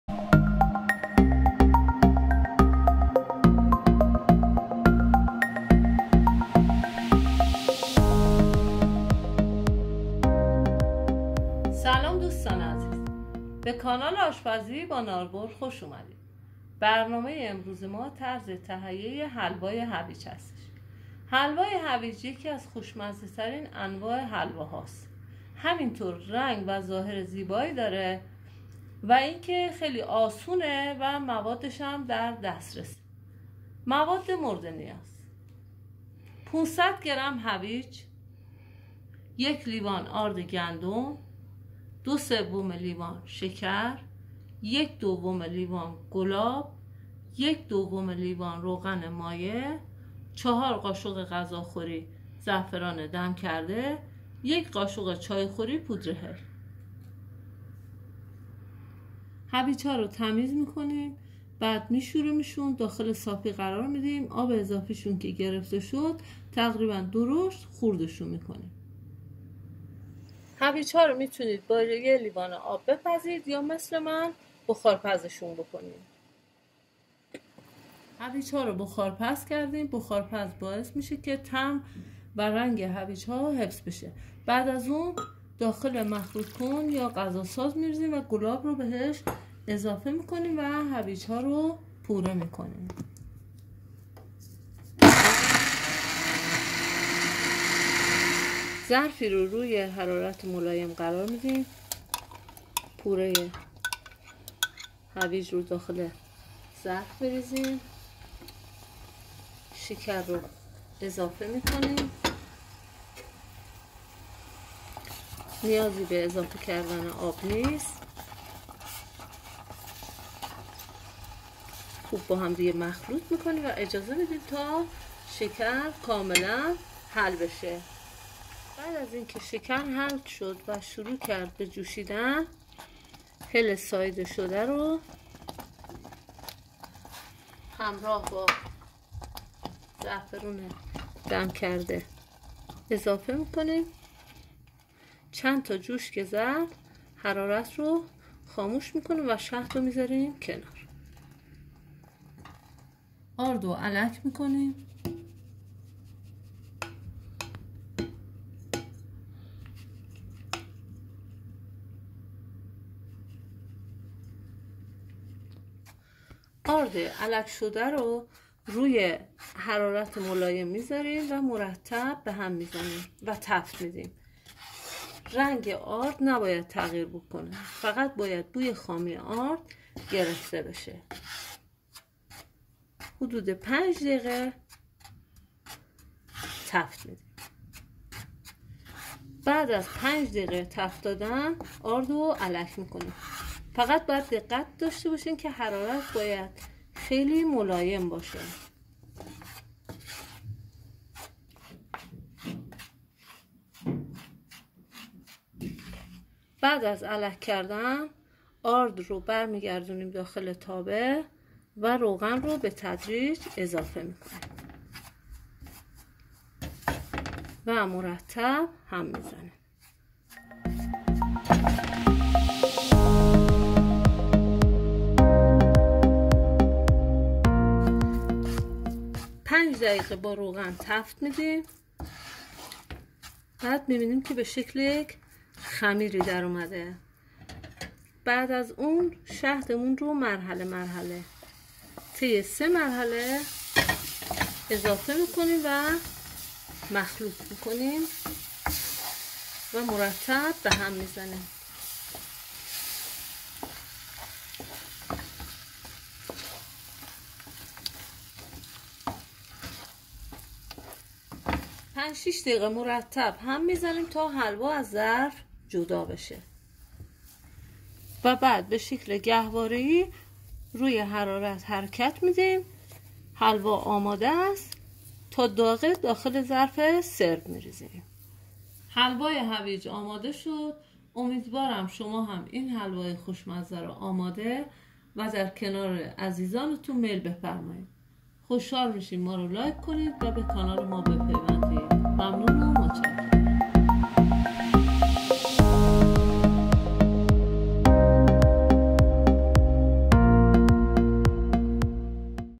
حلوای هویج کم شکر مجلسی‎
یکی از دسرهای خوشمزه ای که می تواند سفره شما را رنگارنگ کند انواع حلوا است، مخصوصا حلوای هویج خوشرنگ و خوش طمع. حلوای هویج یکی از حلواهای پرطرفدار است که می توانید آن را در عرض کمتر از یک ساعت تهیه کنید. حلوای هویج کم شکر هست زیرا که خود هویج شیرین است، به راحتی تهیه می شود و سبک و بسیار خوشمزه است.